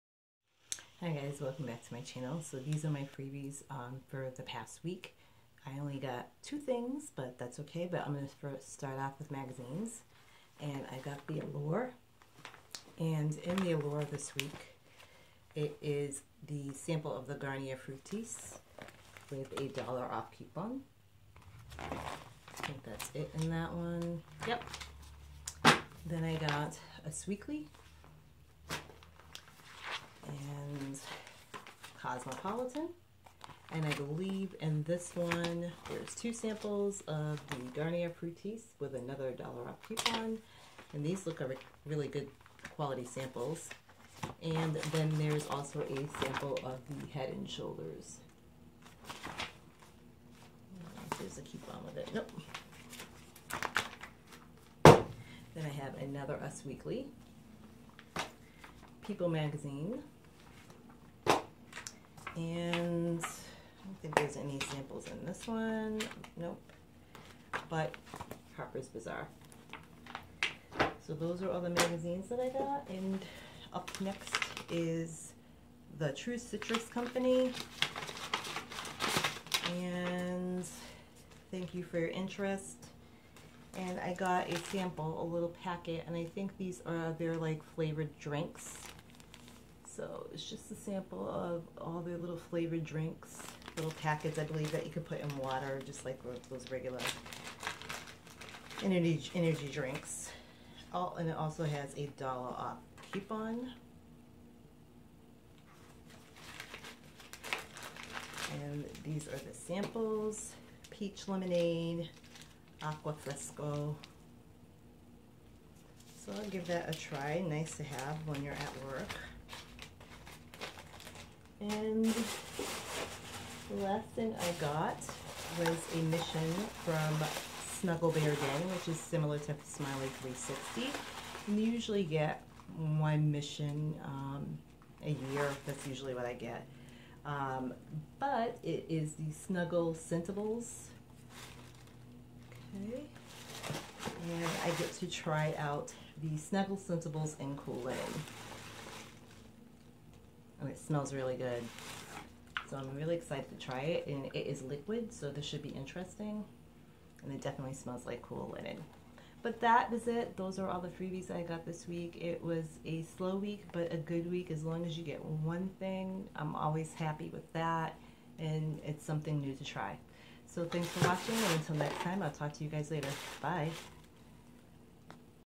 How to get: free items/products in the mail week ending 4/29/17
These are all of the free product samples and free full sized items that I recently received in the mail. This video is to help people that are looking for ways to save money and find great freebies or products to try out by signing up online. Always try before you buy! Below are some of the places where you can sign up to get free items. Please let me know if you have any questions.

I used some of the following websites below to find free items/ products in this video: